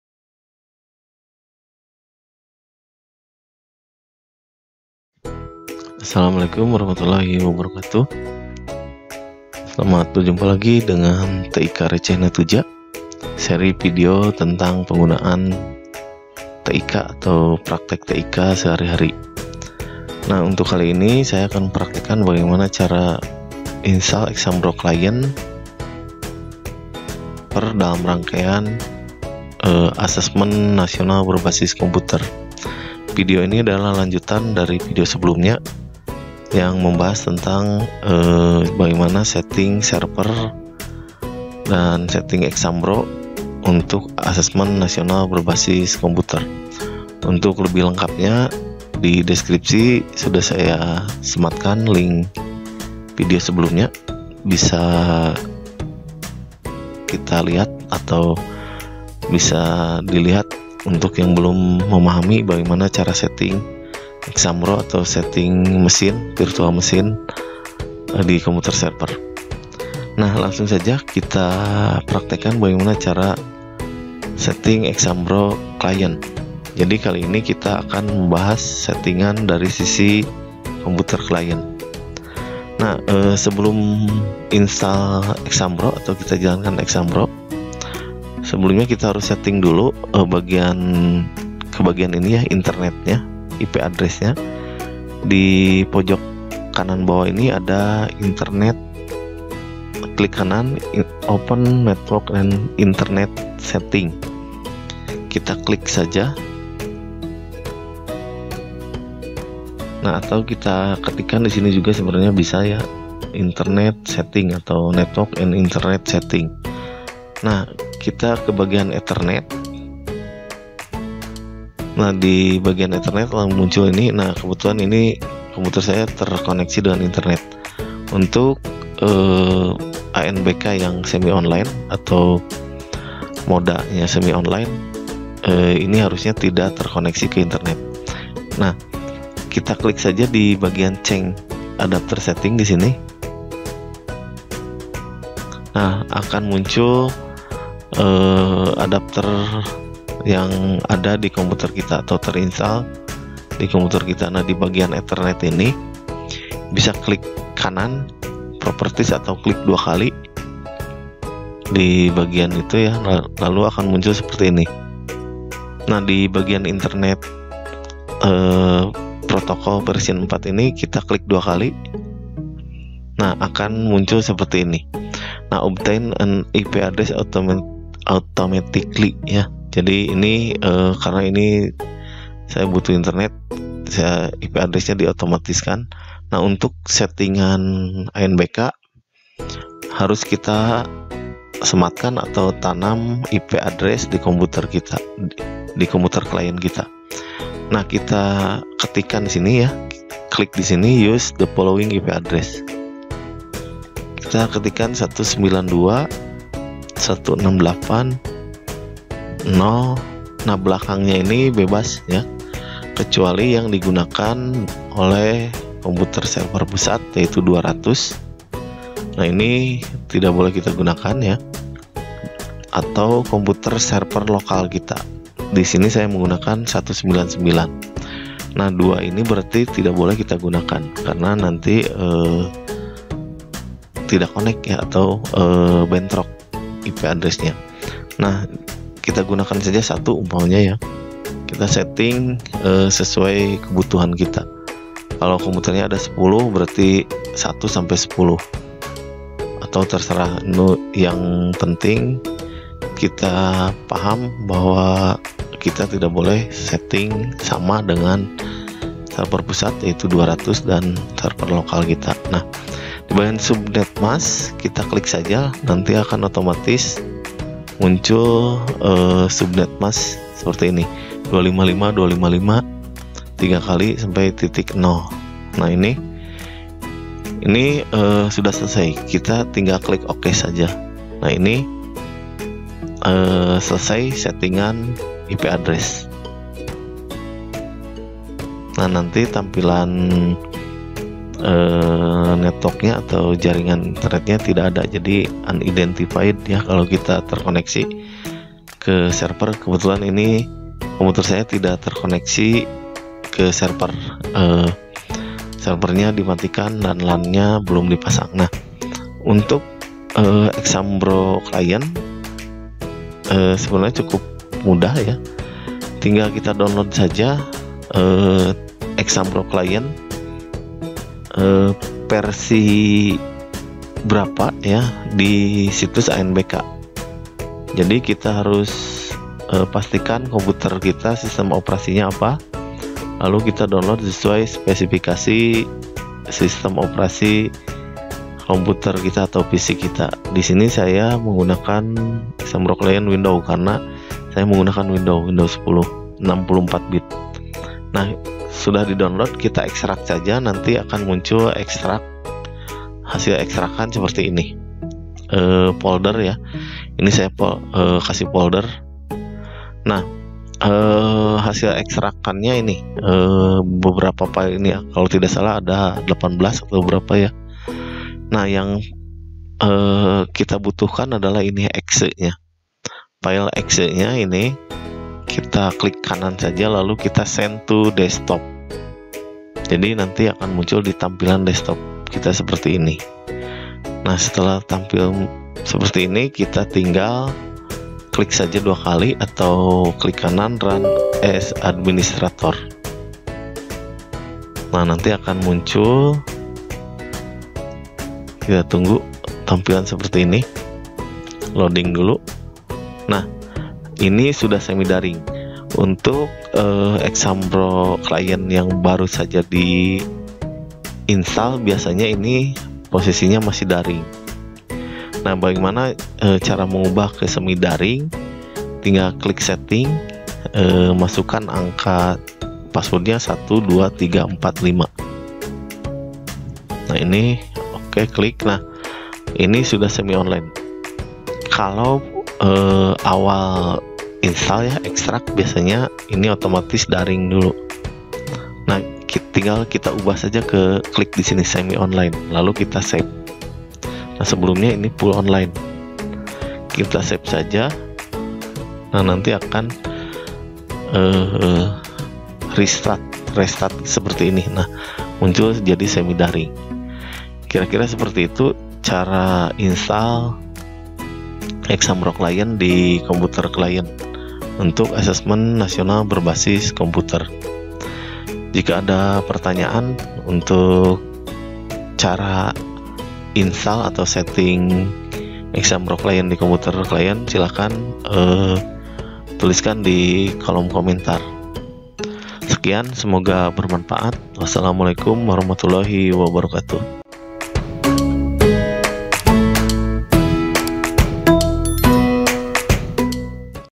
CARA INSTALL EXAMBRO CLIENT
Video ini adalah video lanjutan dari video sebelumnya yaitu cara install exambro proktor di komputer proktor(server ujian), video ini adalah cara install exambro client di komputer client. Video sebelumnya bisa dilihat pada link berikut :